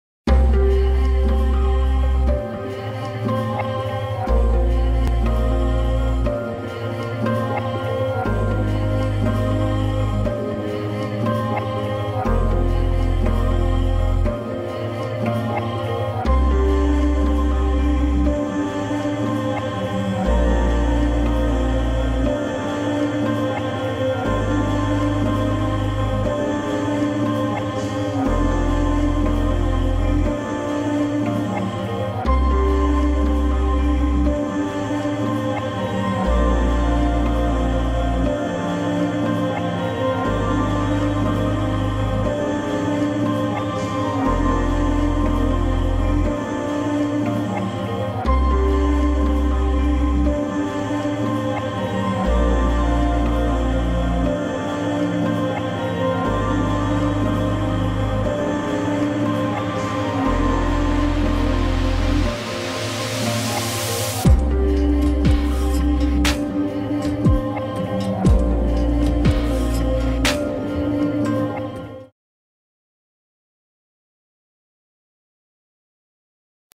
Međunarodni dana epilepsije - Šibensko kazalište zasvjetilo u ljubičastoj boji
Šibensko kazalište večeras je zasvijetlilo u ljubičastoj boji, boji lanande kojom se obilježava Međunarodni dan epilepsije i Nacionalni dan oboljelih od epilepsije.